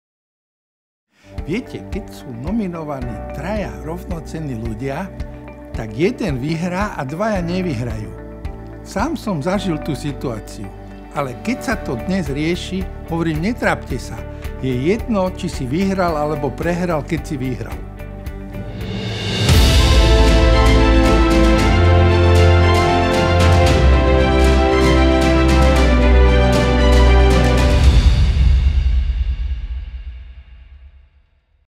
Boris Filan pre Krištáľové krídlo
Člen poroty Krištáľového krídla v kategórii Publicistika a literatúra- spot Krištáľové krídlo TA3. 24.január 2016